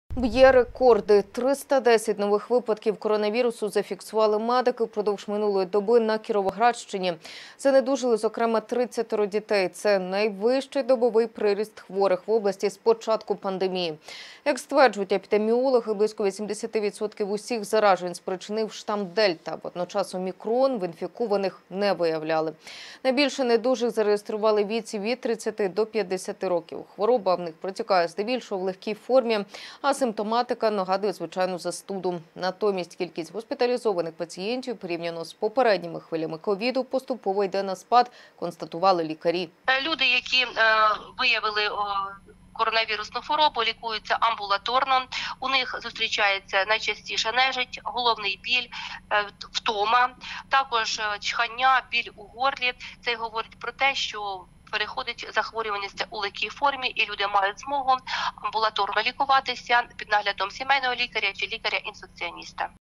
На Кіровоградщині зафіксували рекордну з початку пандемії кількість хворих
258 нових випадків коронавірусу підтвердили медики впродовж минулої доби на Кіровоградщині. Занедужали, зокрема, 22-є дітей.  Це – найвищий добовий приріст хворих в області з початку пандемії. Як стверджують епідеміологи, близько 80% усіх заражень спричинив штам «Дельта». Водночас «Омікрон» в інфікованих не виявляли. Найбільше недужих зареєстрували у віці від 30 до 50 років. Хвороба в них протікає здебільшого в легкій формі. А симптоматика нагадує звичайну застуду. Натомість кількість госпіталізованих пацієнтів, порівняно з попередніми хвилями ковіду, поступово йде на спад